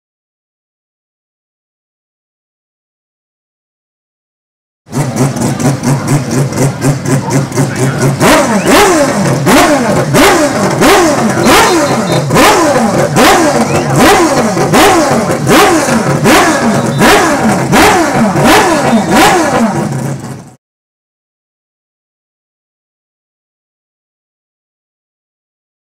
RACER: Mazda 787B Engine Warmup Monterey Reunion 2013
Sometimes a 20-second clip of a 4-rotor Mazda prototype is all one needs to smile. Enjoy this brief clip from the 2013 Rolex Monterey Motorsports Reunion.